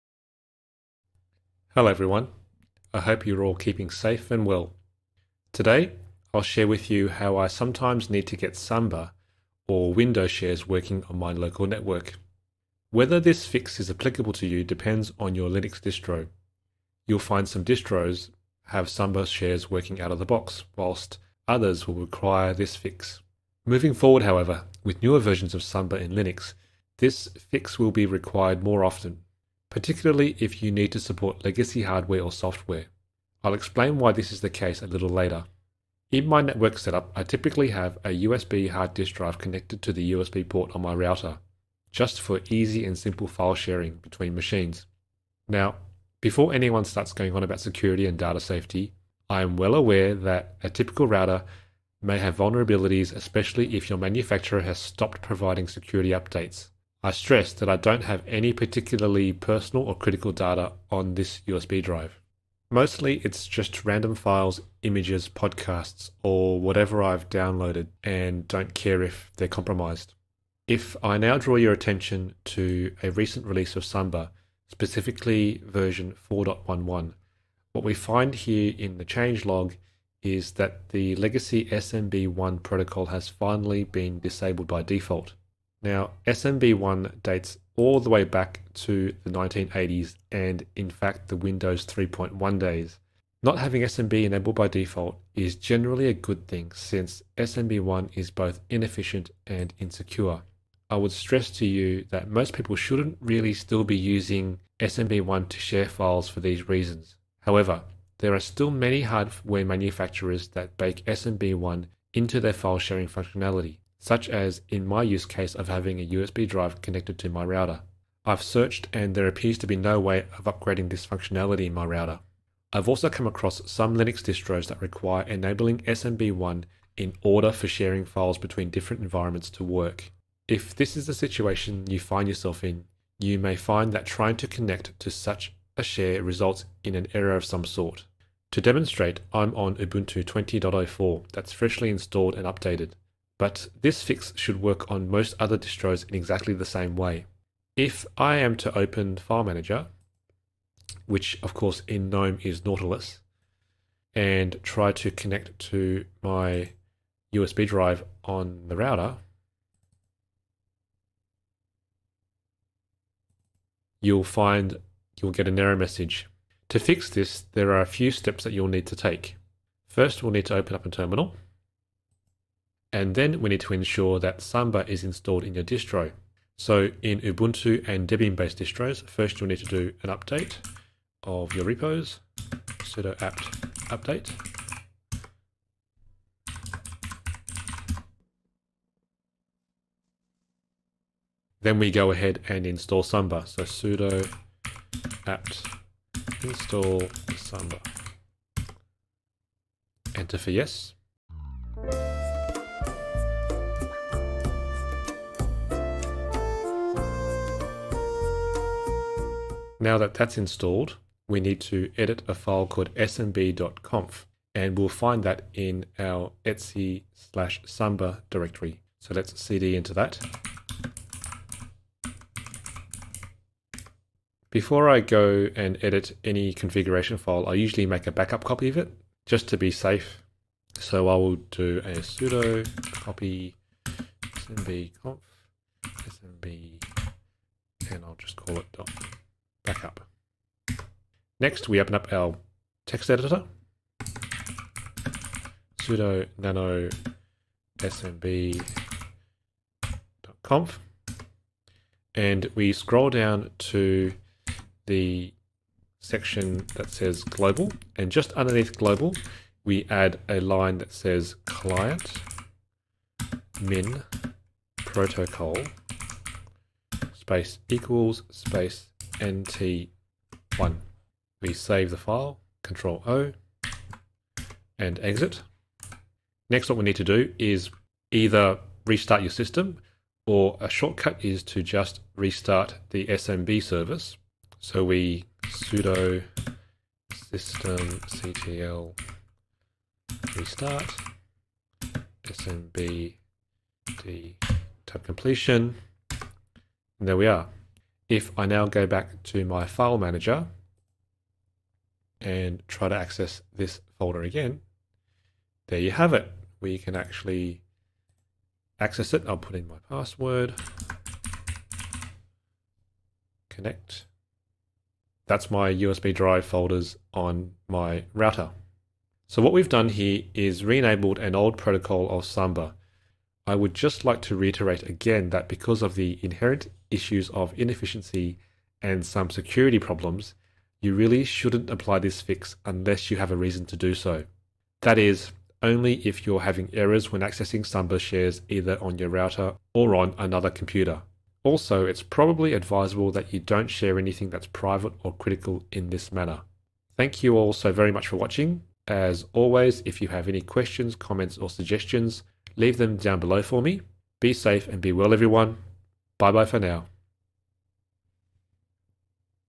Fix Samba (Windows) File Sharing in Linux
How to fix Samba (Windows) file sharing if you are getting errors in Linux. This may occur with older hardware or sharing using your router's USB port.
You may need to enable the older SMB1 protocol. Be careful, however, as this protocol is inefficient, and has security issues.

1. Install samba. In terminal (for Ubuntu and Debian base):
sudo apt install samba

2. Backup your smb.conf file:
sudo cp /etc/samba/smb.conf /etc/samba/smb.conf.bak

3. Edit your smb.conf file:
sudo nano /etc/samba/smb.conf

4. Scroll down to the [GLOBAL] section and add this line:
client min protocol = NT1

5. Restart your computer or restart the smb service:
sudo systemctl restart smbd.service

Attributions:
network-cable-1243146 - Photo by doctor-a from FreeImages